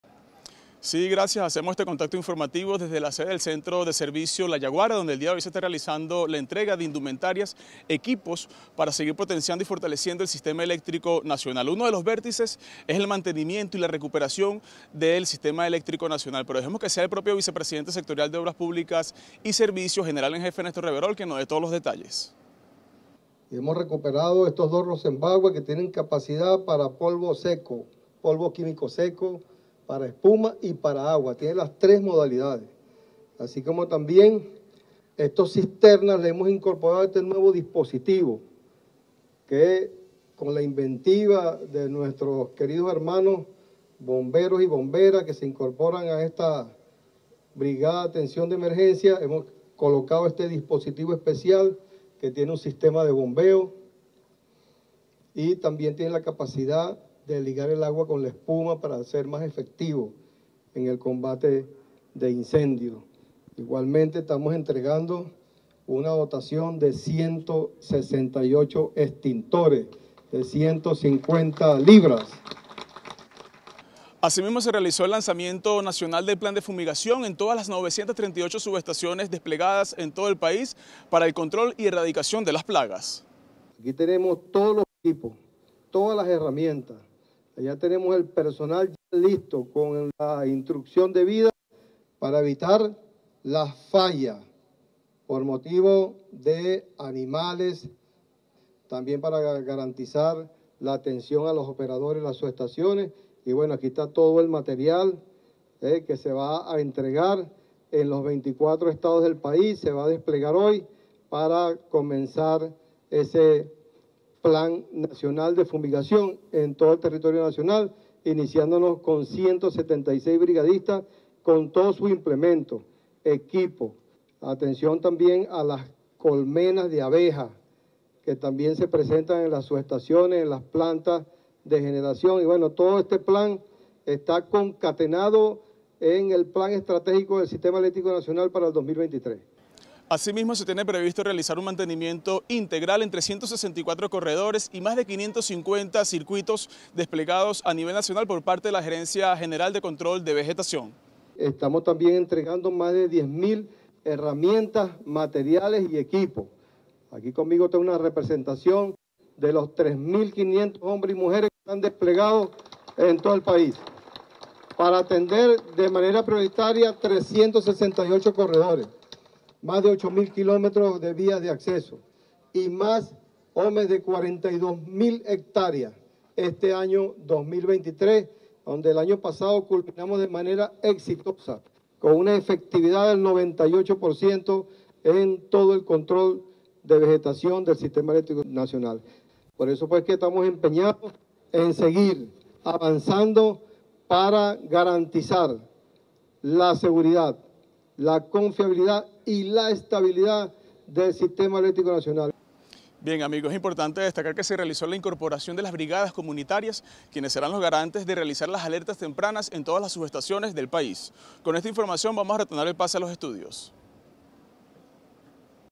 CONTINUIDAD AL MANTENIMIENTO Y RECUPERACIÓN DEL SISTEMA ELÉCTRICO NACIONAL
Hemos desarrollado una actividad muy productiva junto a nuestra fuerza trabajadora de Corpoelec, para dar continuidad al mantenimiento y recuperación del Sistema Eléctrico Nacional, contemplado en la línea N°3 del Plan Estratégico del Ministerio del Poder Popular para la Energía Eléctrica para el 2023.

La Gerencia General de Programación y Control de Vegetación recibió 10 mil 514 equipos, herramientas e insumos para ejecutar el mantenimiento de 368 corredores de servicios de líneas de transmisión, 800 kilómetros en vías de acceso y 559 circuitos de distribución.

También, activamos el Plan Nacional de Fumigación para atender las 938 estaciones, subestaciones nodales y plantas de generación priorizadas del país, contando para esta tarea con 176 brigadistas que se desplegarán en 44 equipos de trabajo con especialistas en control y erradicación de plagas.

En materia de seguridad y prevención realizamos una importante dotación de vehículos y equipos a la Brigada de Control de Emergencias, que incluye 2 unidades Rosenbauer con tres versiones: agua, polvo seco y espuma, 13 camionetas Hilux, 3 cisternas de 8 mil y 6 mil litros de agua, 5 mil esferas de extinción y 164 extintores de 150 libras.

Igualmente, se suman a este equipo 100 hombres y mujeres del Poder Popular organizado para conformar la Brigada Comunitaria Voluntaria, que alertará sobre alguna eventualidad en las subestaciones del Distrito Capital.

¡Seguimos trabajando para el fortalecimiento de los servicios públicos en el país!